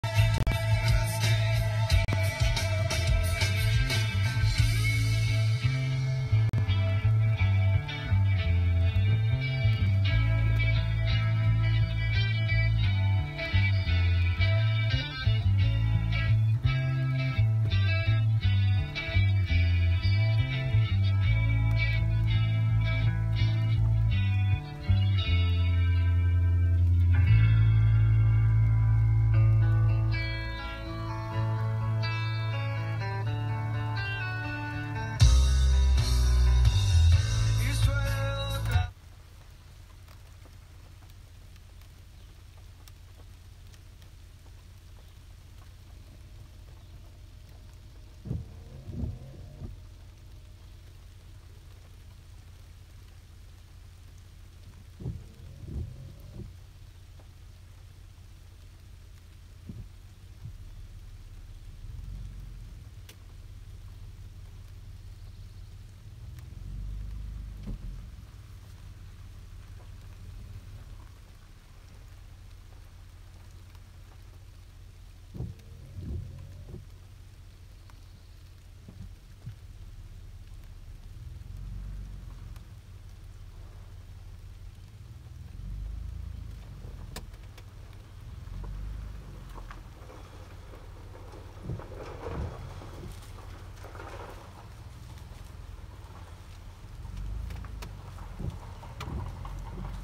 Can You Spot The Deer?
Just a quick video of a deer, staring at the truck.
I think there are three of them altogether.
You see them a lot more in the spring than any other time of year.
And they don't run away...Do they know it's not hunting season?  ;D